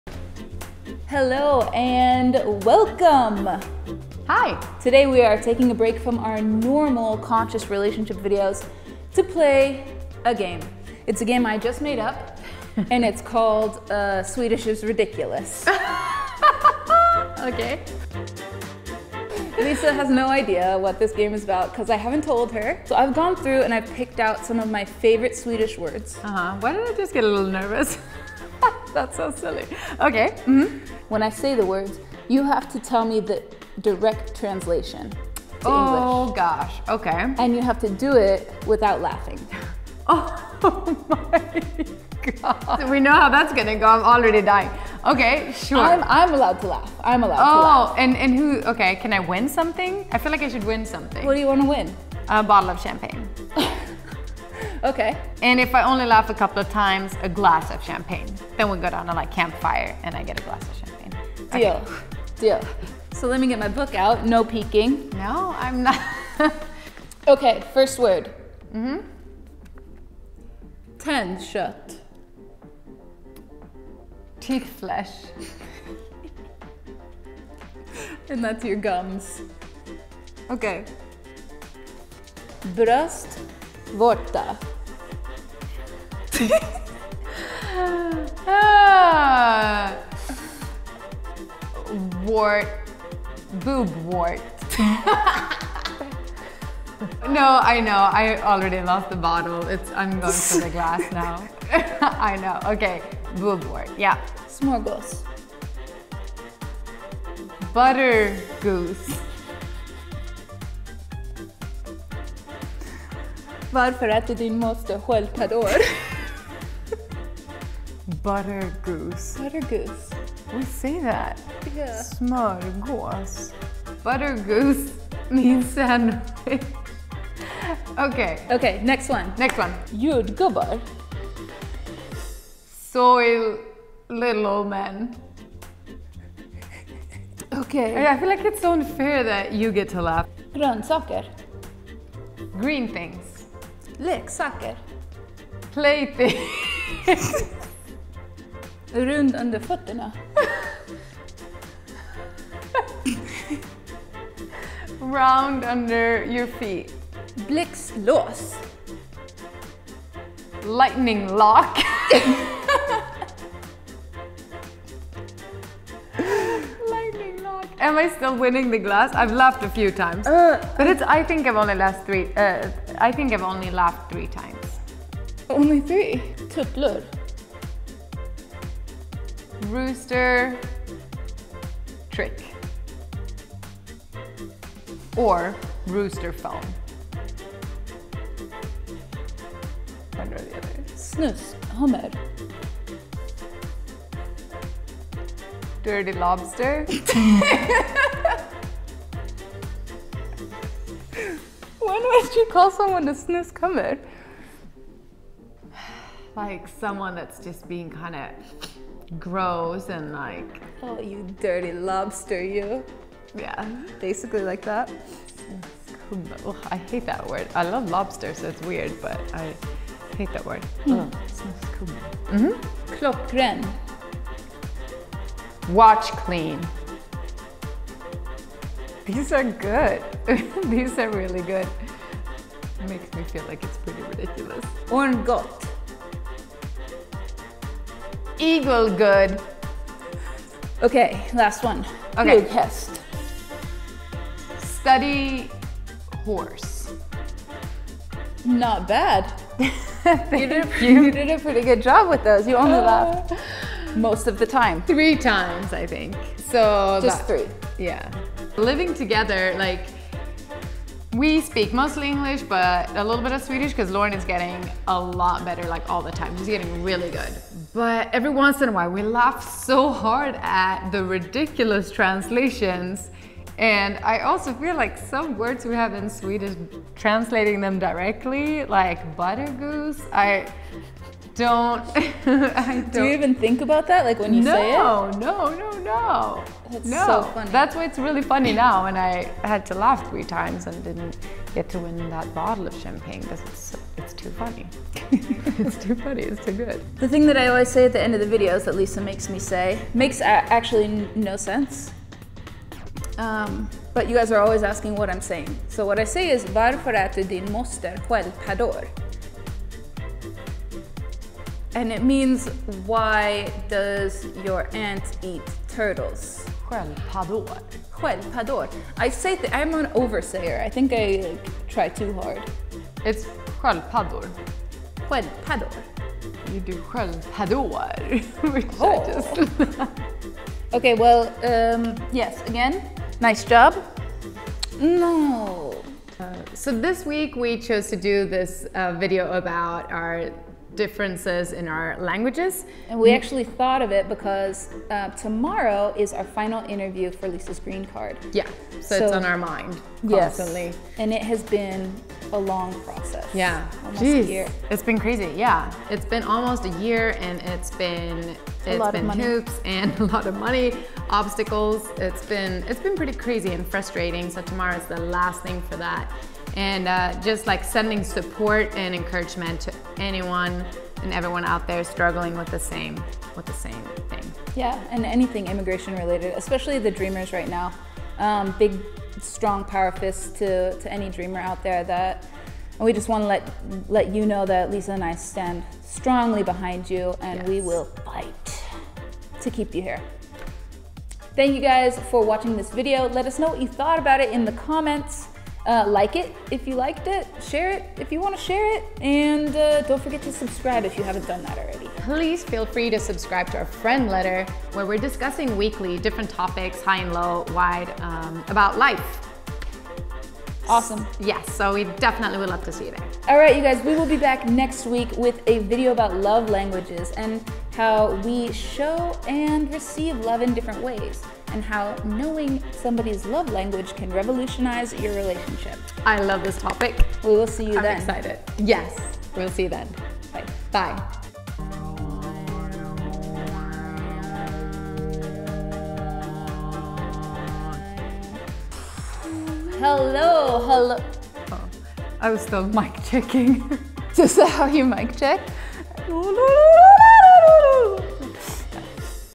Try Not to Laugh Challenge: American Makes Swedish Wife Translate Words to English
“Can I win something? I feel like I should win something.”

 This week we decided to do something a little different and do a funny language video. As I’m learning Swedish, I’m constantly giggling to myself about different funny words in the Swedish language that if taken literally, mean something really hilarious. I gathered some of my favorites and surprised Lisa with a challenge video. We can’t watch this without laughing, can you?

If you were really looking forward to a relationship video this week, don’t worry, we’ll be back with a good one next Wednesday ;)

Logo: Ellie Morello

Instagramz
Lauren: @buttoncake
Lisa: @lisahallingmusic
Us: @thiscolorfulworld